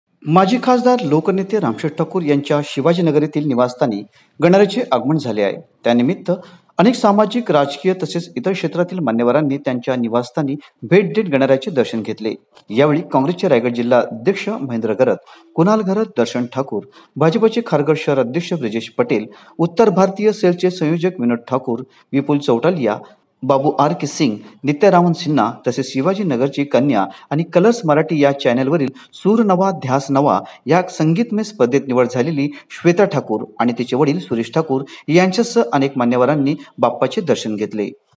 लो.रामशेठ ठाकूर यांच्या शिवाजीनगर येथील निवासस्थानी गणरायाचे अनेक मान्यवरांनी घेतले दर्शन !
लोकनेते रामशेठ ठाकूर यांच्या शिवाजीनगर येथील निवासस्थानी गणरायाचे अनेक सामाजिक ,राजकीय तसेच इतर क्षेत्रातील मान्यवरांनी घेतले दर्शन !